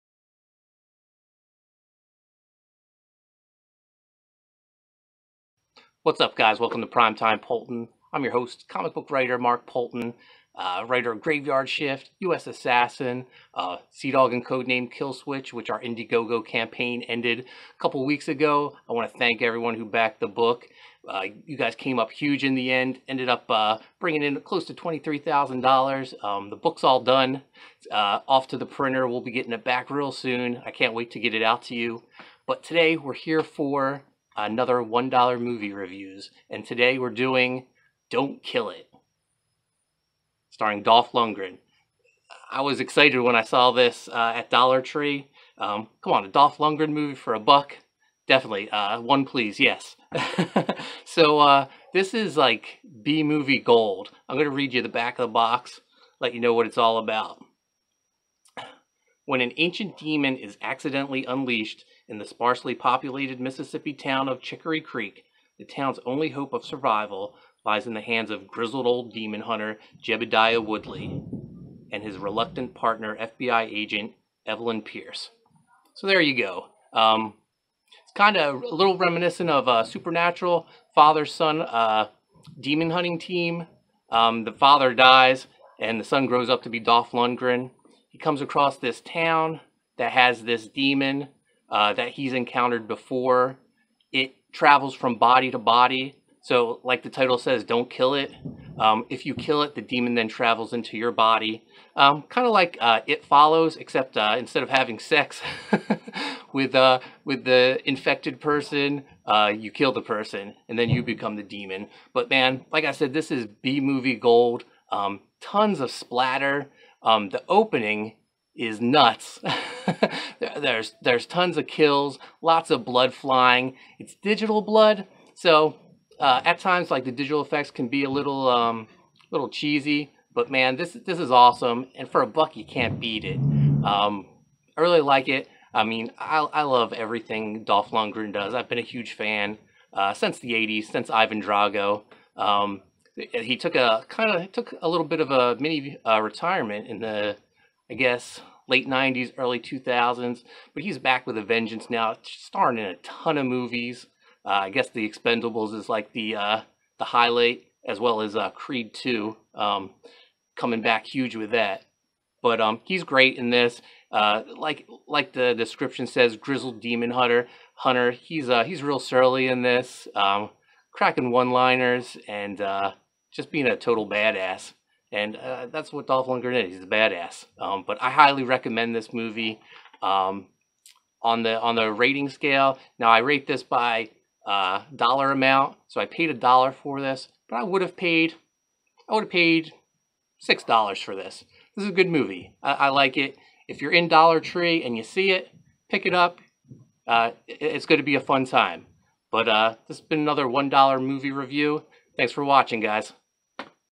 $1 Movie Reviews - Don't Kill It
Comic book writer, Mark Poulton (Savage Hawkman, Graveyard Shift) reviews his latest Dollar Tree movie pick up, the Dolph Lundgren horror movie, Don't Kill It!